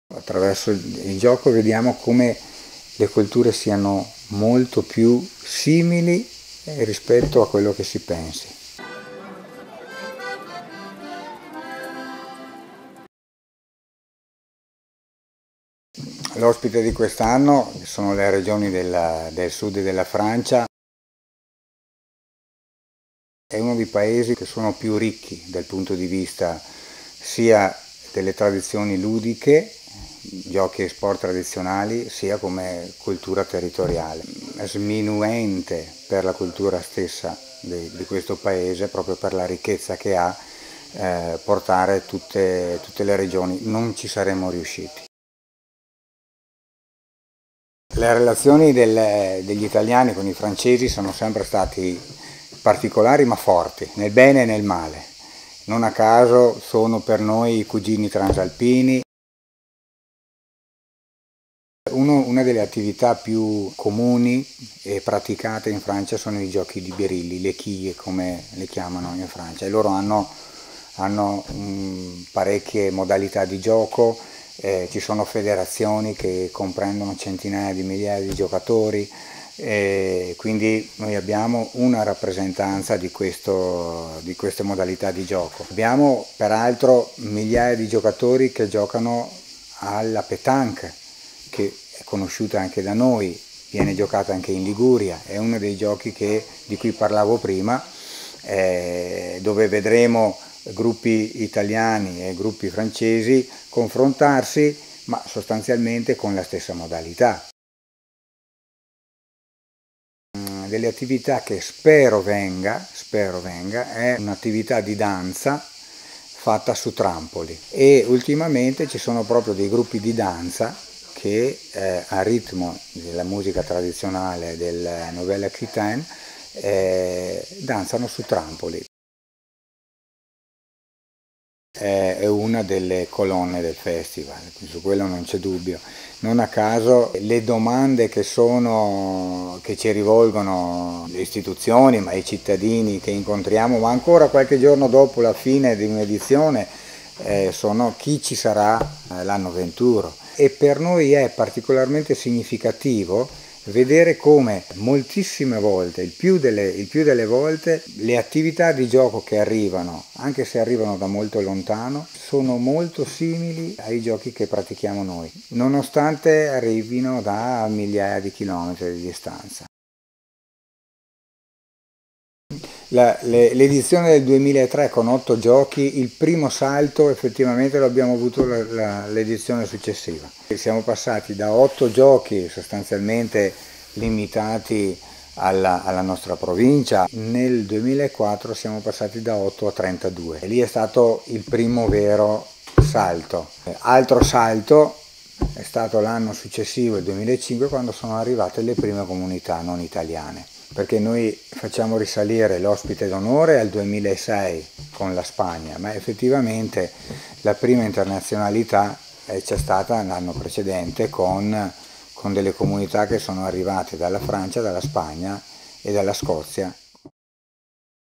Tocatì 2018 - Le voci del Festival - Paolo presidente Aga
Abbiamo fatto una bella chiacchierata con Paolo, presidente di AGA, che oltre a rivelarci il paese ospite (che non è un vero paese) e a darci qualche anteprima su quello che questa comunità porterà al festival, ci racconta qualche chicca sulla storia del Tocatì. Per esempio chi di voi si ricorda la prima edizione del 2003 con 8 giochi?